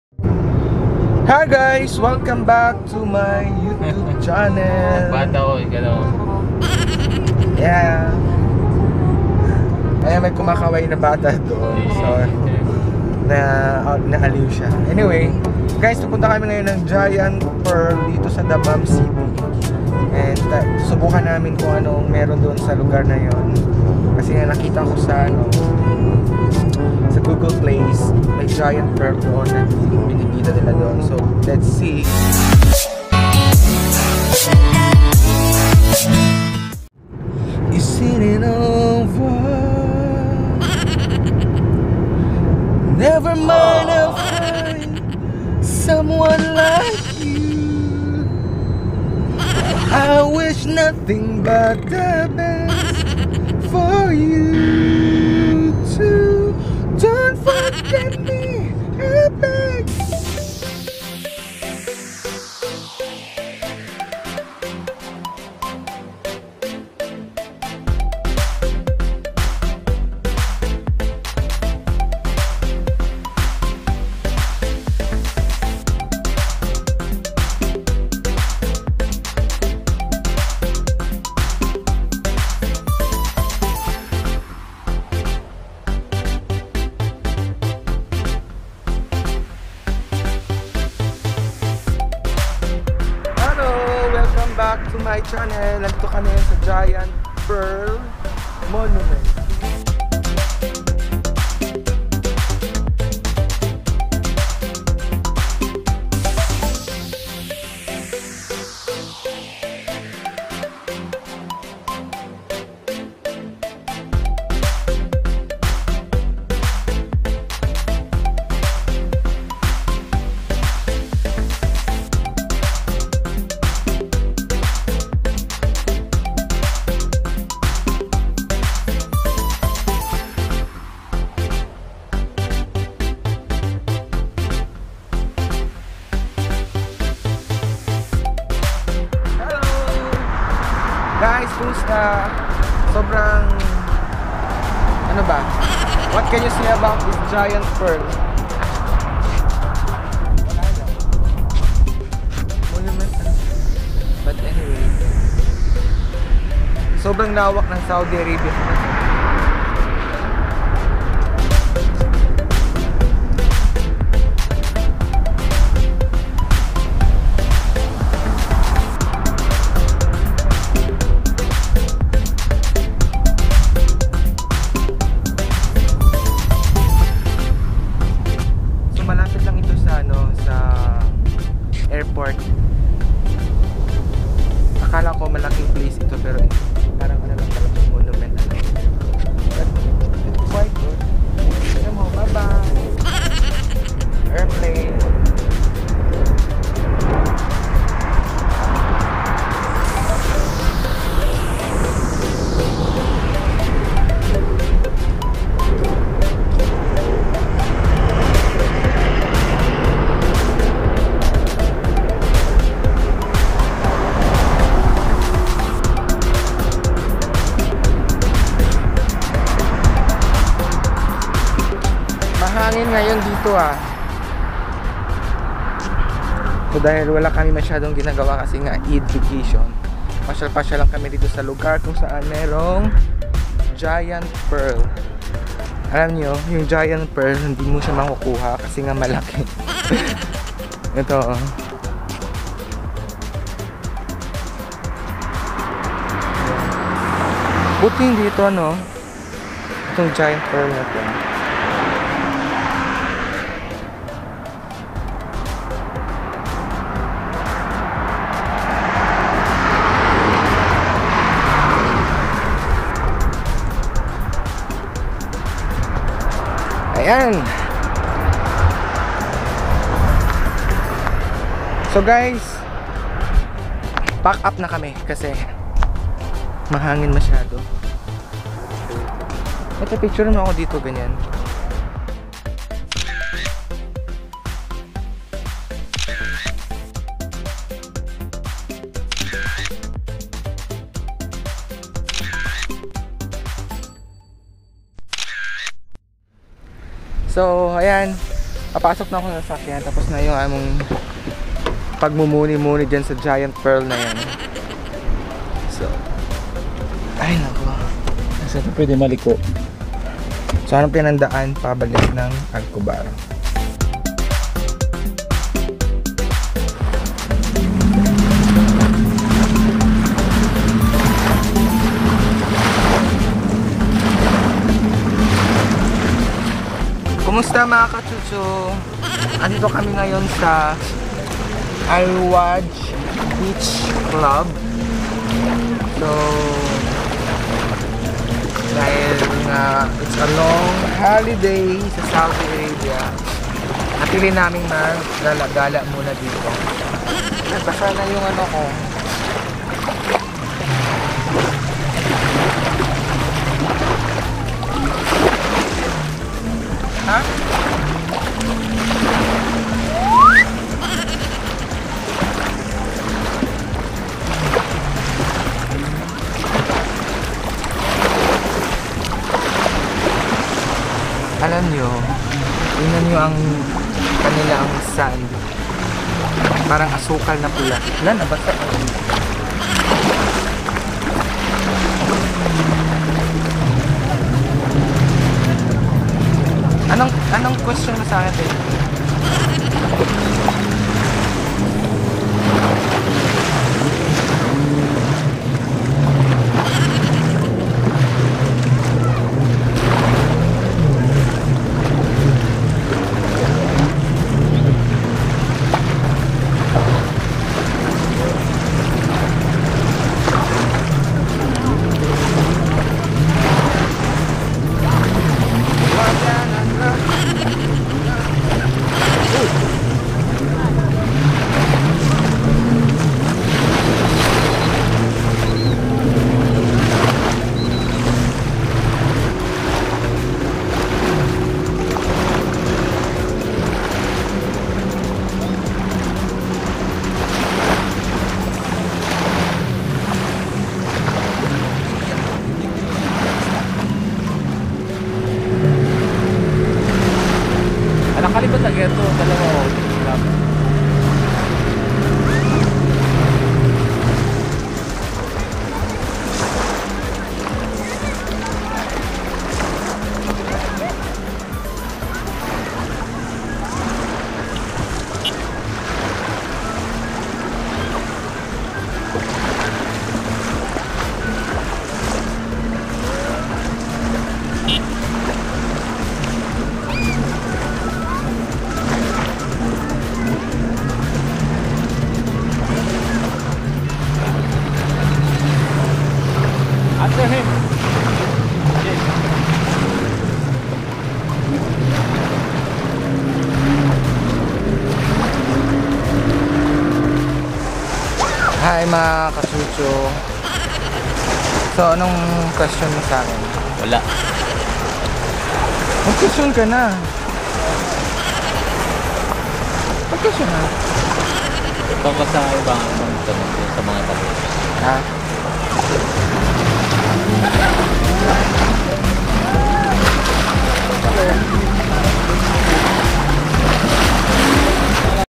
Ang higanteng Perlas ng Gitnang Silangan
Episode 33: Nag lakbay kami para ma saksihan ang higanteng perlas dito sa Dammam City.